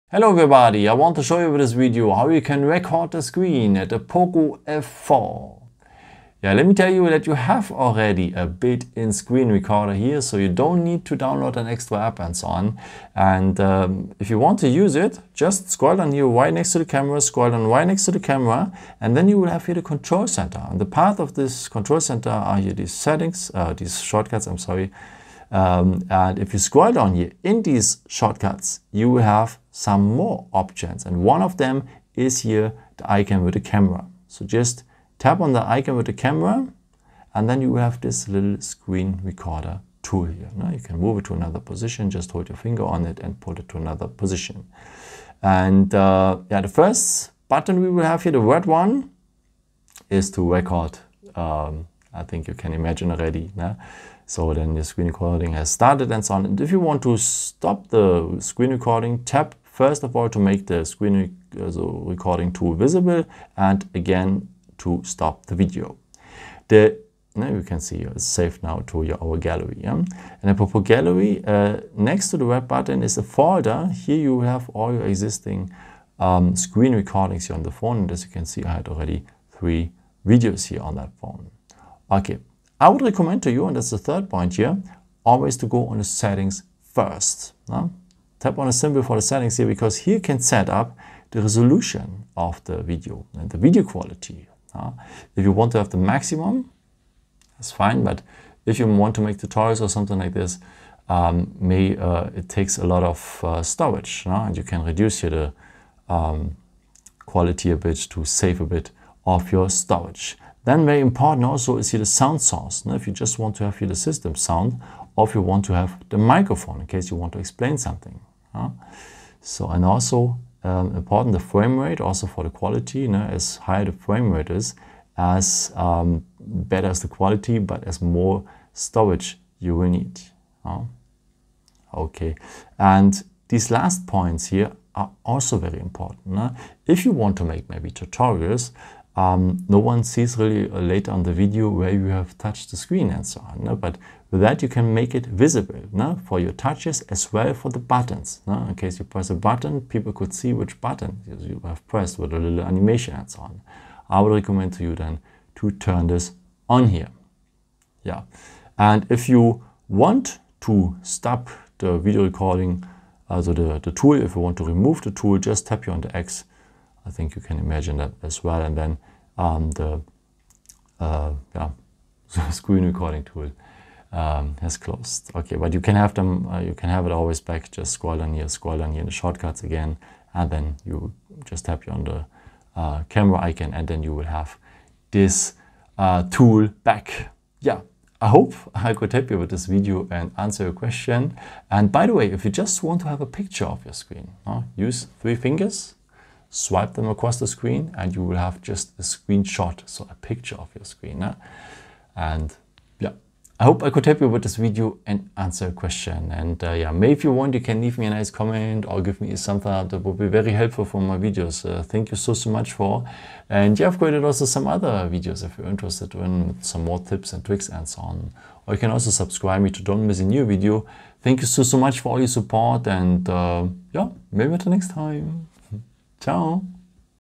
Xiaomi Poco F4: How to record the screen? take a screencast | screen recording
With this video, I want to show you, how you can record the screen at the Poco F4 5G by Xiaomi. With that build-in screen recorder you can record your actions on the screen.